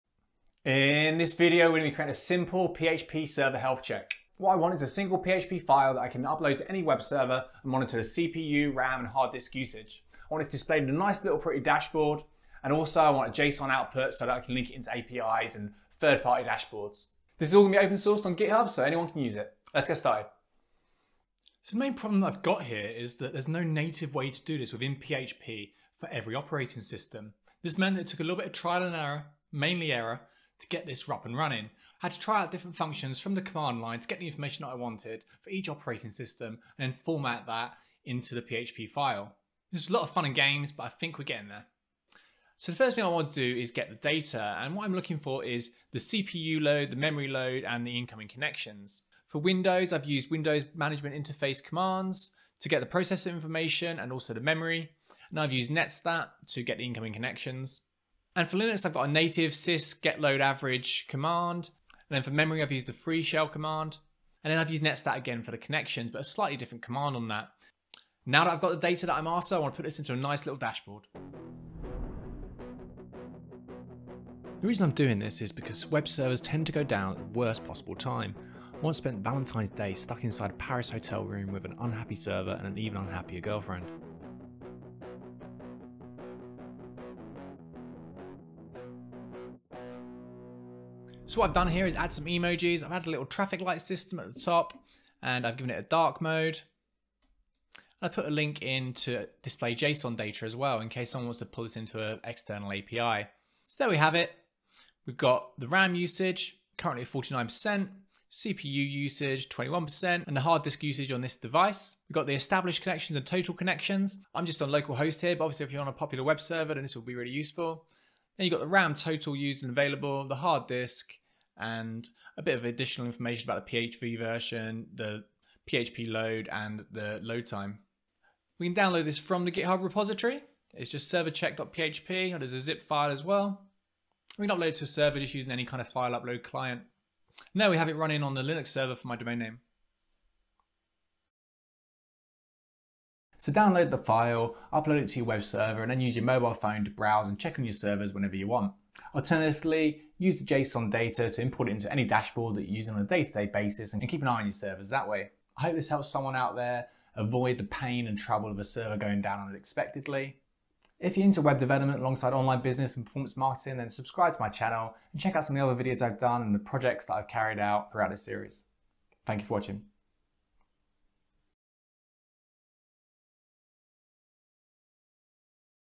Creating a PHP Server Check Script (CPU, RAM, HD)
For this project I wanted to create a little single file script that I can drop into any web server and use it to keep an eye on the CPU RAM and Hard Disk Usage